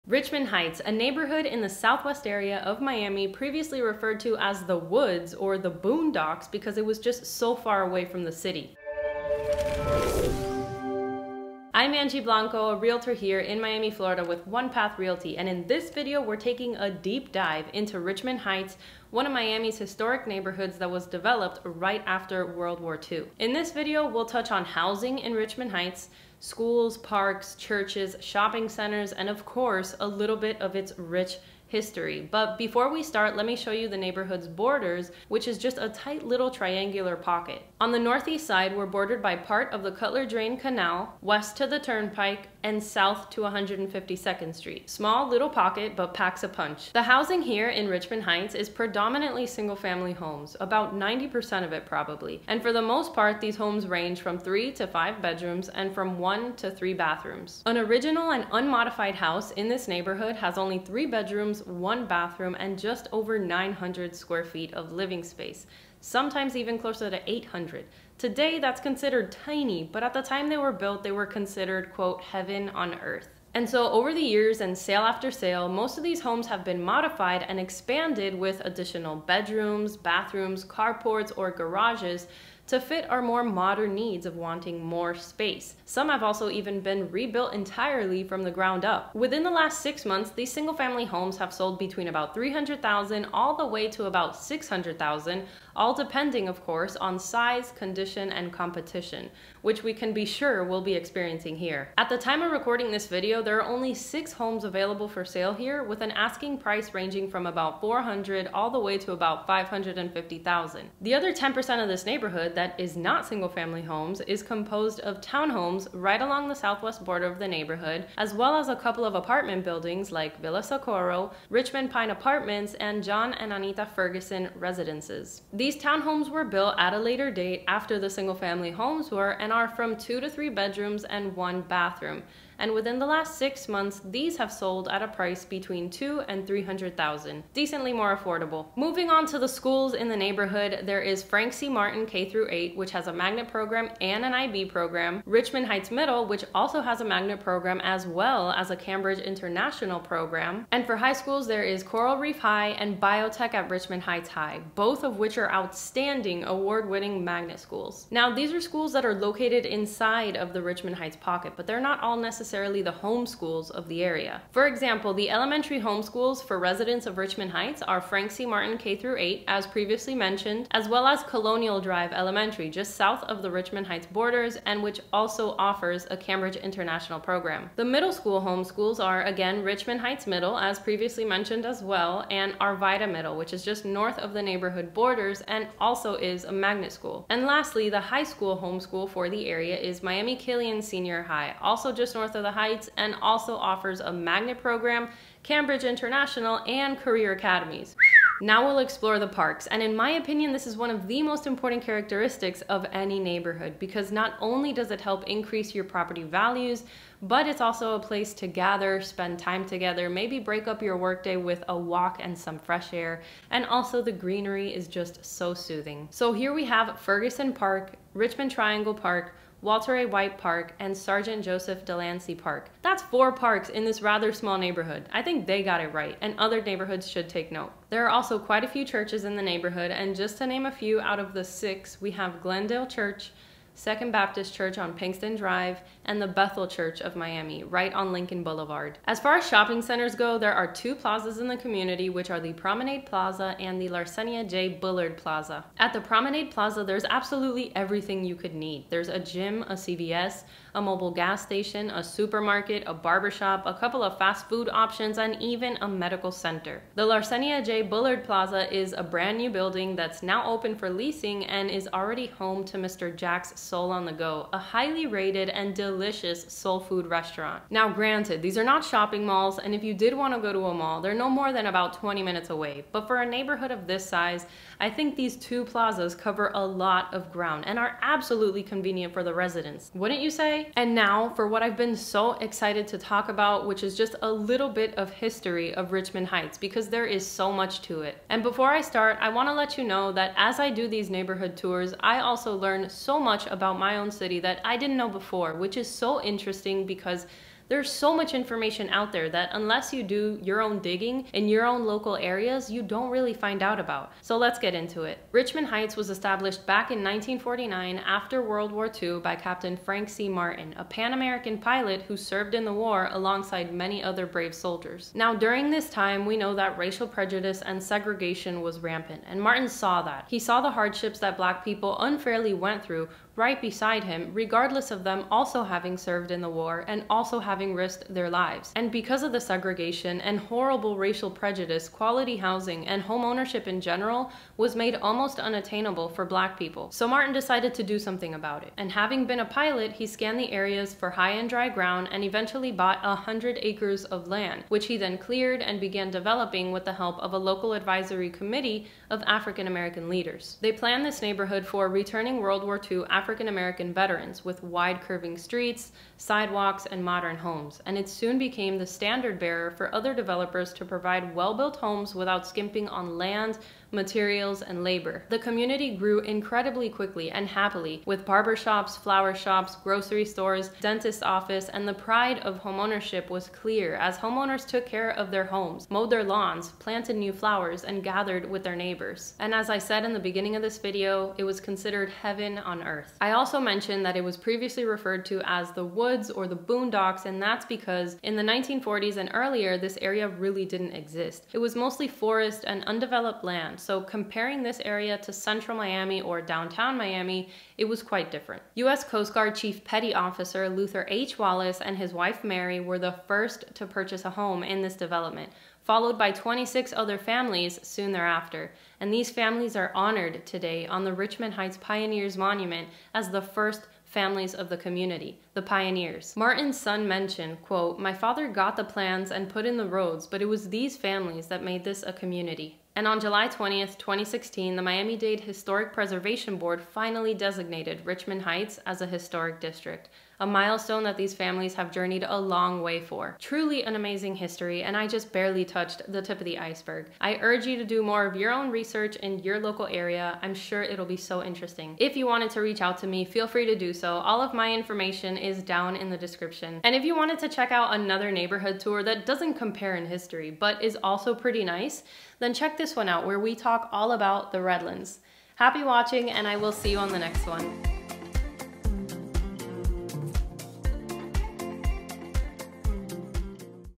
Richmond Heights, Florida | Historic Miami Neighborhood
Richmond Heights is a neighborhood in Miami that is FULL of history. In this video, we take a dive into Richmond Heights' real estate and home prices, schools, parks, churches, shopping centers, and of course - a little bit of its rich history... because there is so much to it.